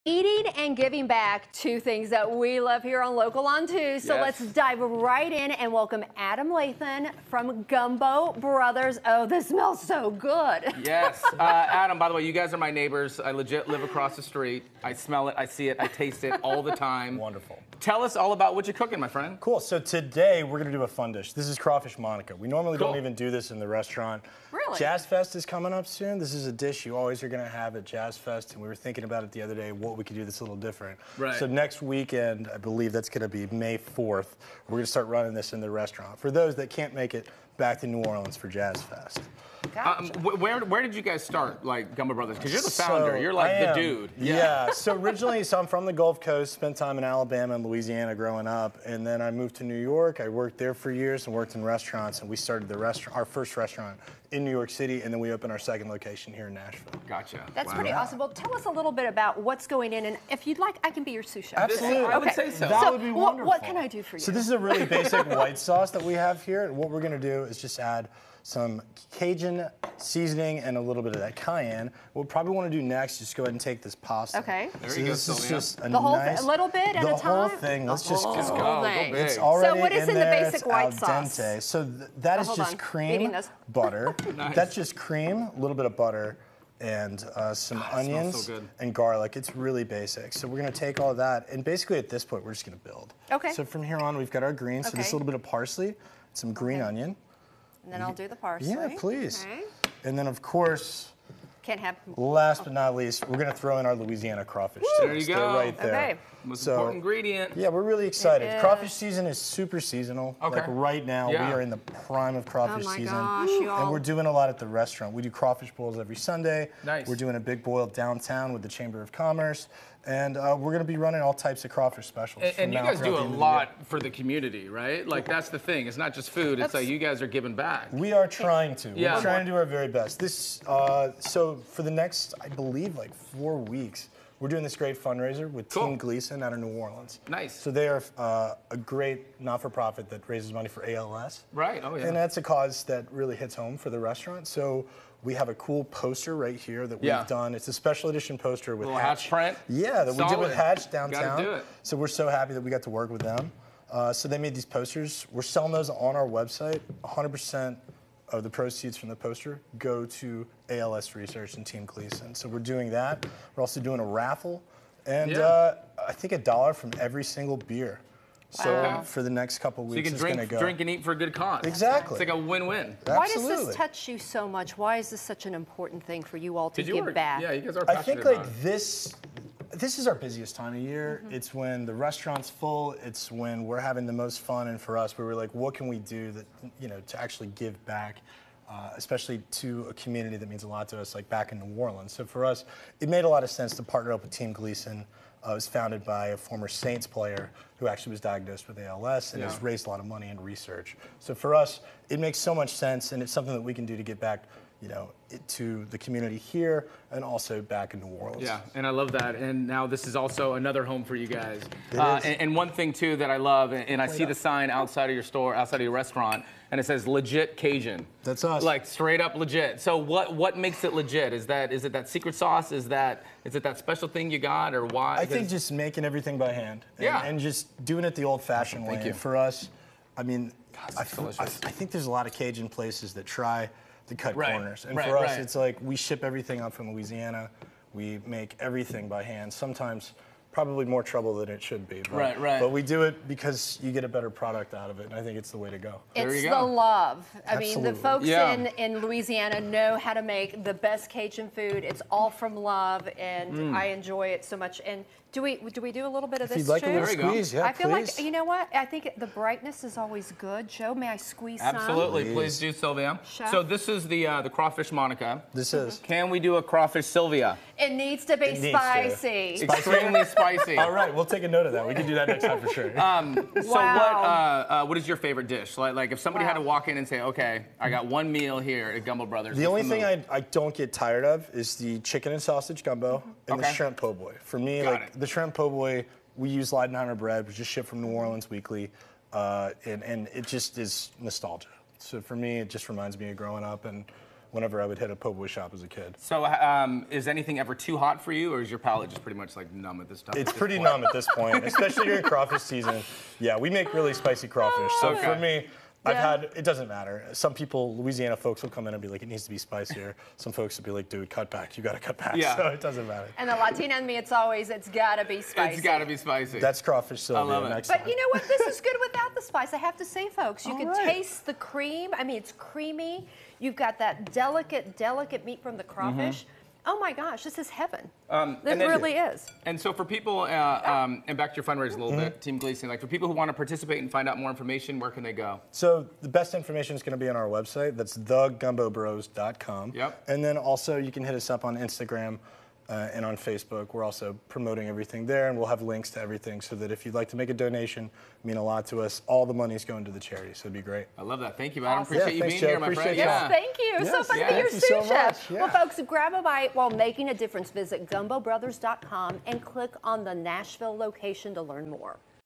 The Gumbo Bros: Make a difference with every bite
The Gumbo Bros joined Local on 2 to share how you can give back while enjoying some great food.